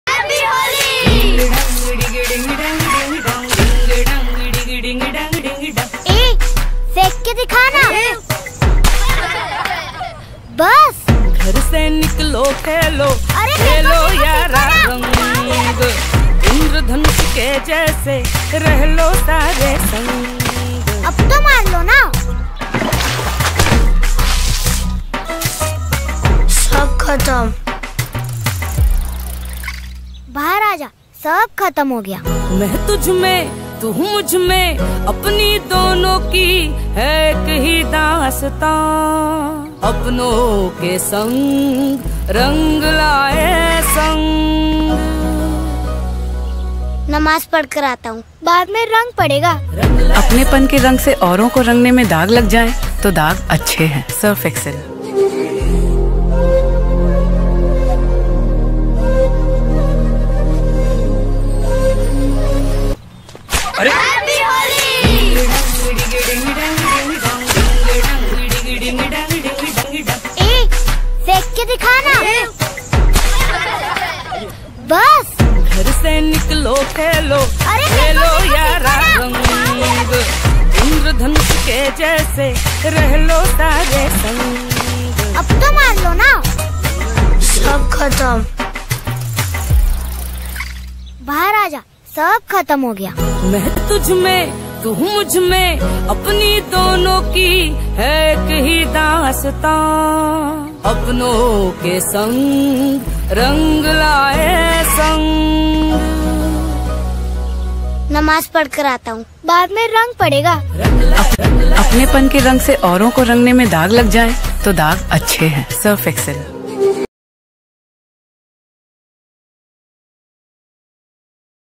Virtual Team Building Activity - Surf Excel Ad - ISSM Business School
Virtual Team Building Activity performed by the Students of ISSM Business School, Chennai.

First Year MBA Students of ISSM Business School has performed & Recreated the Surf Excel Ad in this Pandemic with Social distancing.

Virtual Management Activities are performed by the Students to develop their Management & other Skills.